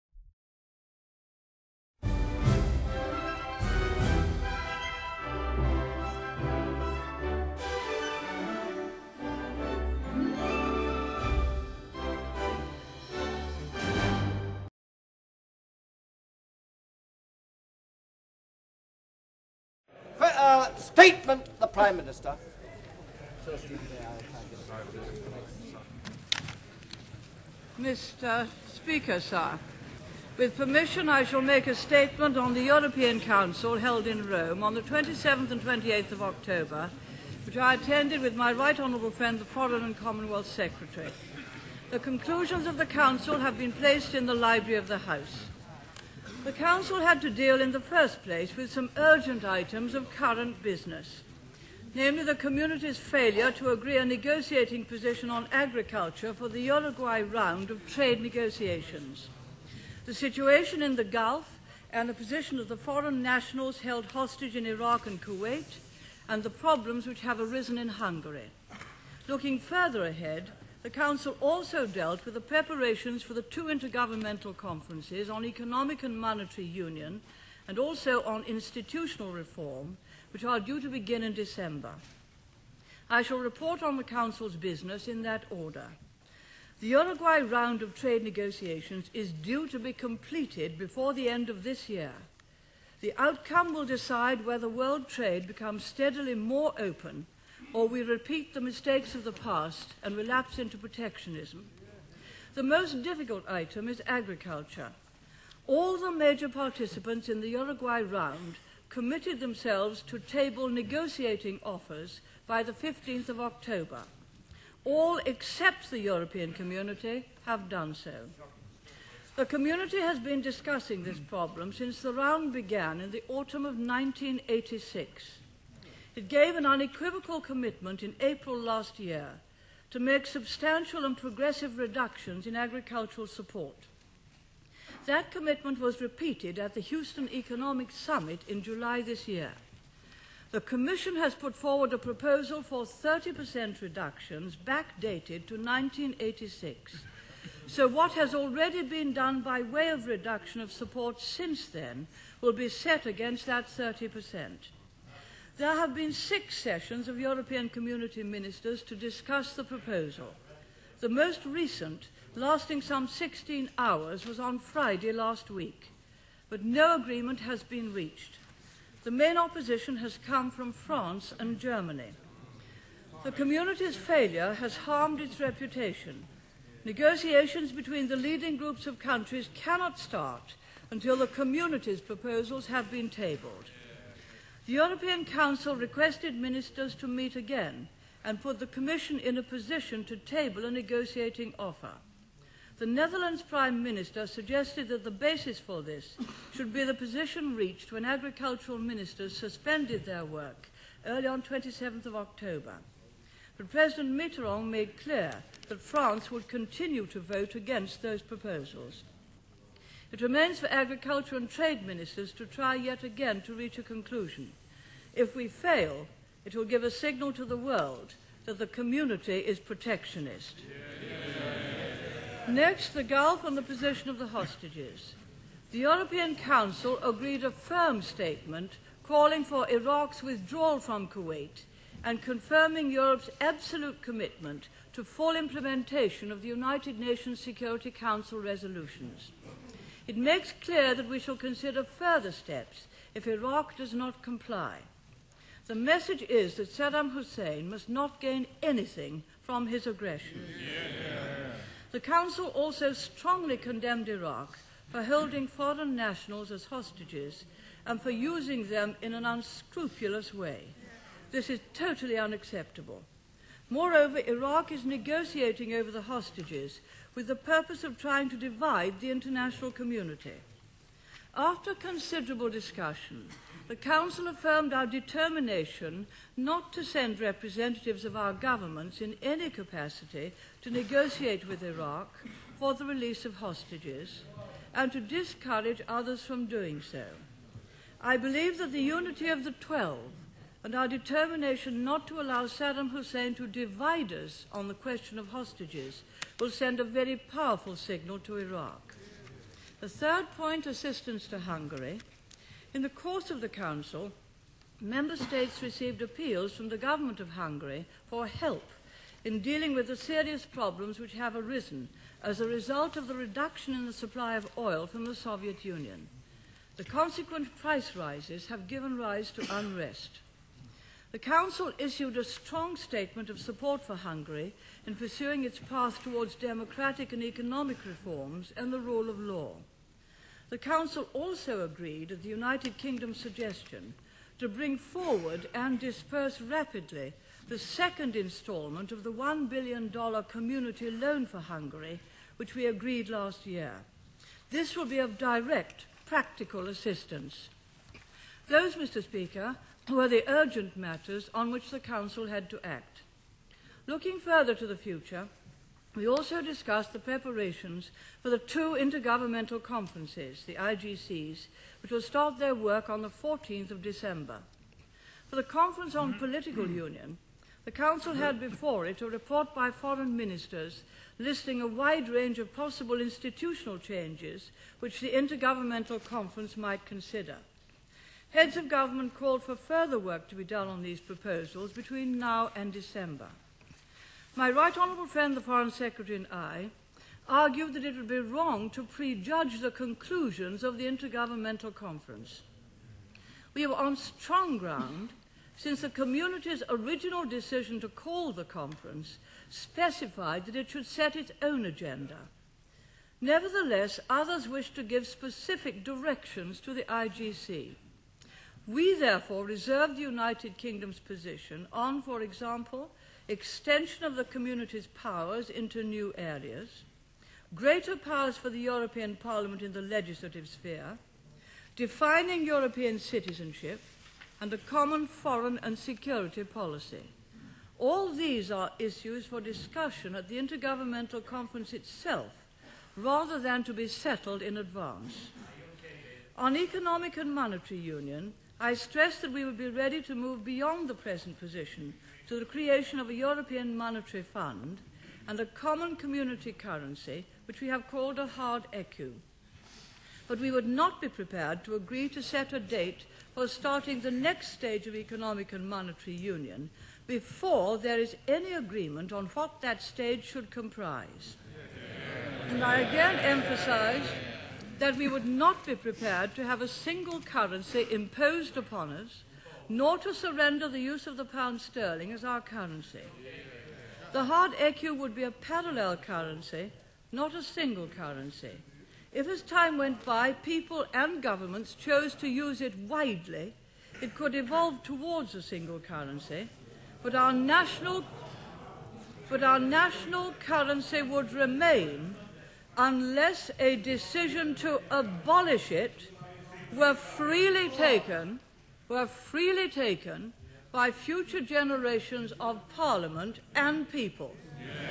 Margaret Thatcher Statement on Europe No! No! No! Part 1
the famous statement on the EU council where maggie stood up for the UK and said no! no! no! sadly this performance helped lead to her own downfall.